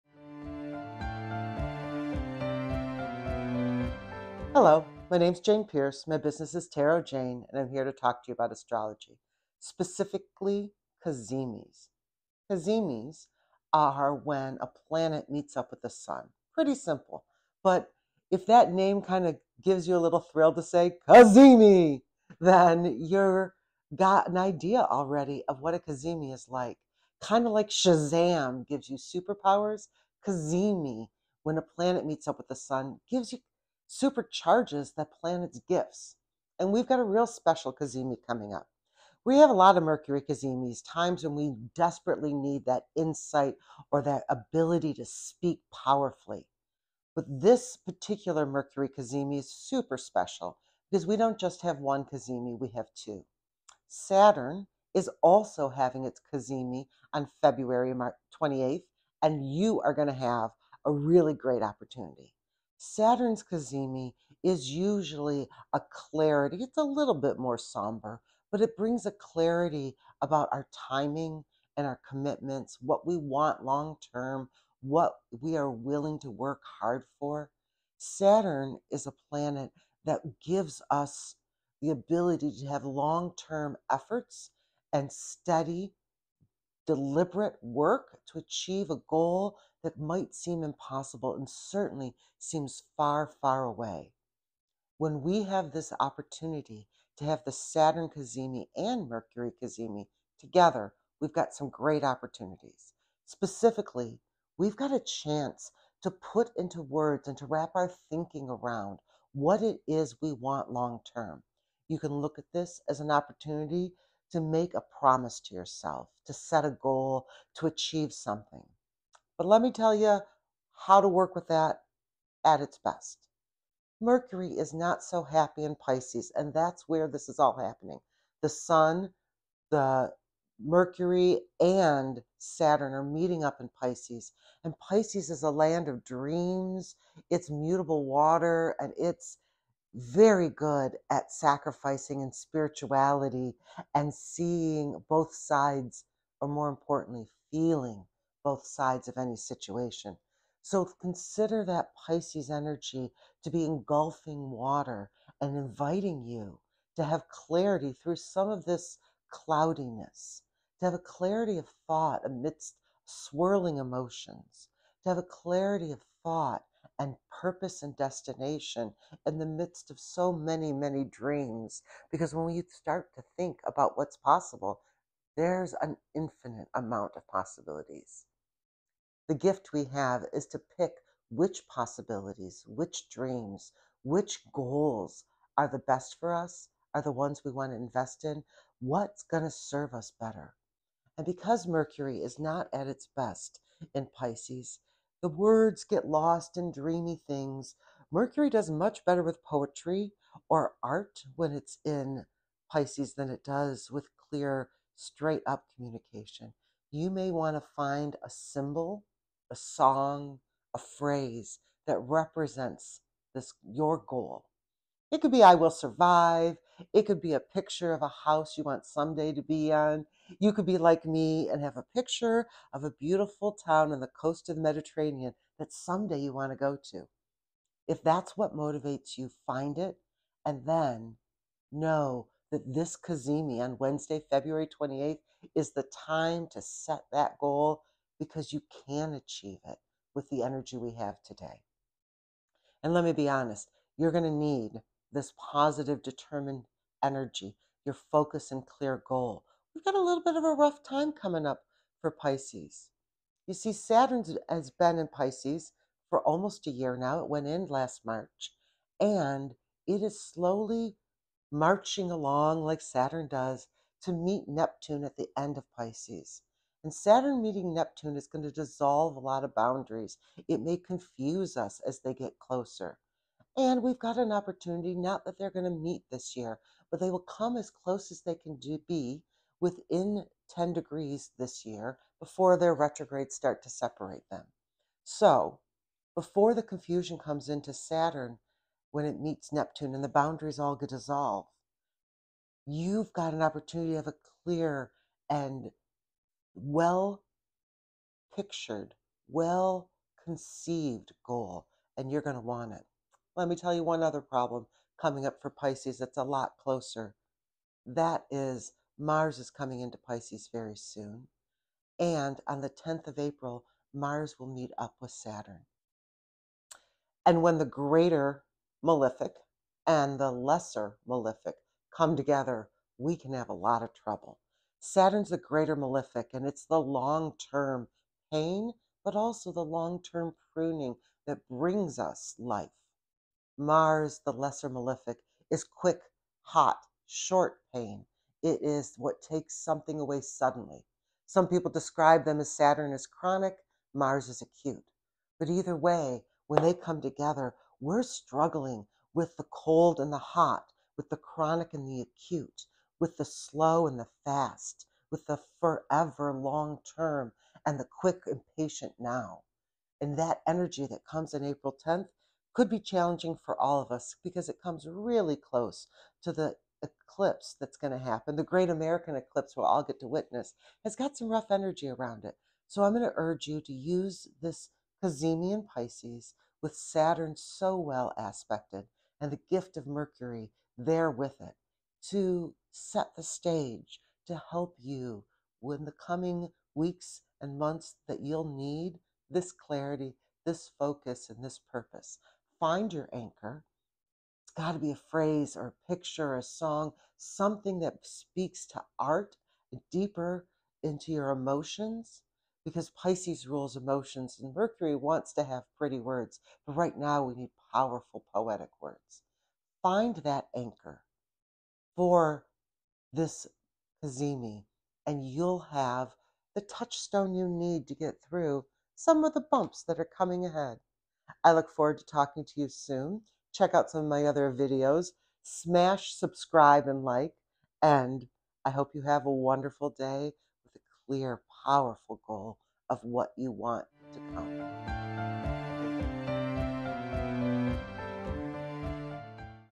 Supercharging Your Dreams with the Double Cazimi
Cazimi! It sounds a little like Shazam! If you shout it will you become a superhero? During a Cazimi you might feel like it. When a planet gets a supercharge from meeting the Sun the gifts of that planet get stronger. This week we have both Mercury and Saturn getting that boost during the double Cazimi.

The Mercury Cazimi is shinning it's light in Pisces, the land of dreams. Usually the Cazimi brings a burst of clarity and the beginning of a new cycle of communication that focuses on the area of our life and the topics associated with the Sign where these planets meet. In Pisces we focus on belief and goals but might find Mercury is more artistic than clear. Use the power of Piscean emotions to pick a symbol, song or affirmation that stirs you to action instead of the usual list that Mercury loves to make.
Instead of a normal Mercury Cazimi, this one happens on the same day as the Saturn Cazimi. The yearly Saturn Cazimi brings a clarity about our boundaries and sets the new cycle for the discipline we need to make things last. Decide what you value before Pisces sees some stormy waters in March and April.

Original music composed and performed by Henry